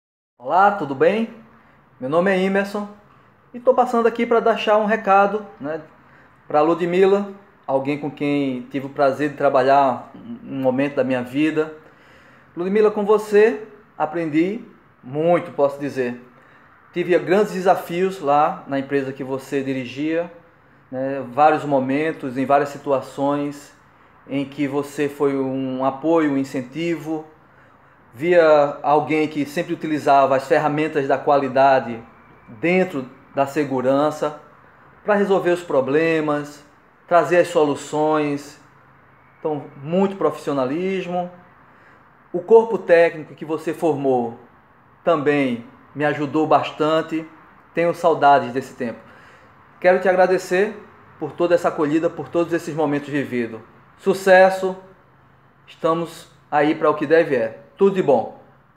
Depoimento II- Imerson Almeida
Imerson  foi um técnico que me ajudou muito, sempre paciente, calmo e muito negociador. Um profissional diferenciado que foi relevante na equipe técnica das construções dos navios com muita qualidade e segurança do trabalho.

Que bom que pude agregar valor a sua caminhada meu amigo. E obrigada por compartilhar abertamente a mais pessoas.

Que possamos voltar um dia a dividir experiências.

Conte comigo. Sucesso!